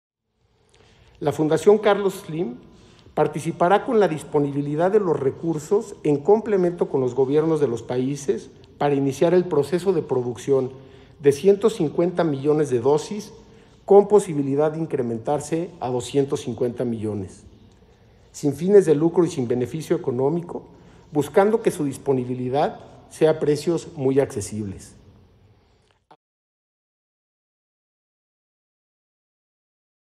Fundación Slim promete de 150 a 250 millones de vacunas contra el coronavirus en Latinoamérica
La fundación del magnate mexicano Carlos Slim anunció que producirá entre 150 y 250 millones de dosis de la vacuna contra el coronavirus con el laboratorio AstraZeneca y las distribuirá en América Latina "a precios muy accesibles".